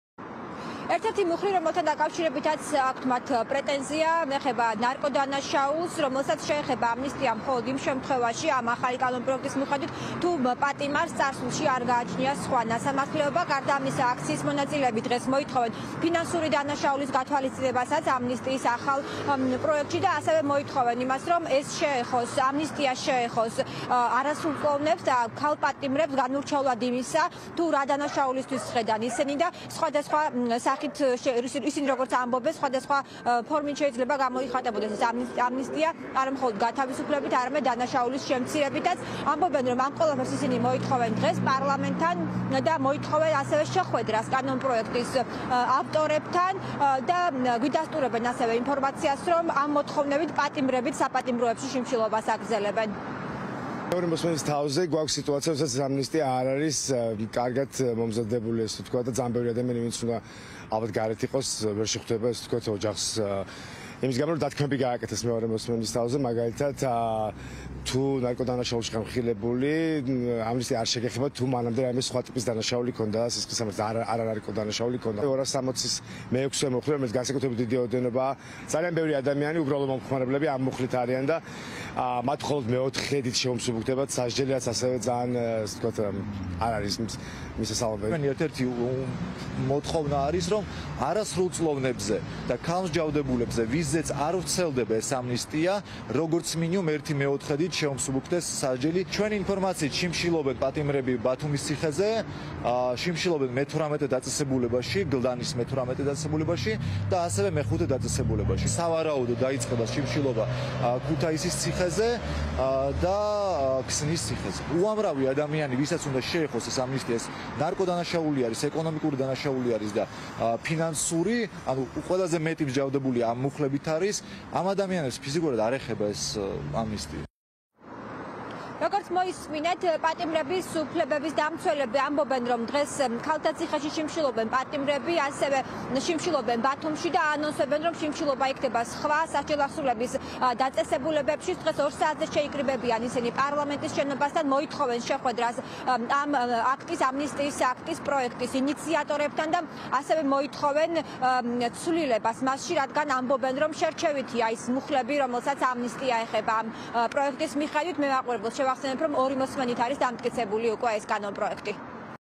პროტესტი ამნისტიის შესახებ
▪️ ამნისტიის შესახებ „ქართული ოცნების" კანონპროექტს დღეს პატიმრების ოჯახის წევრები და ინტერესების დამცველები გააპროტესტებენ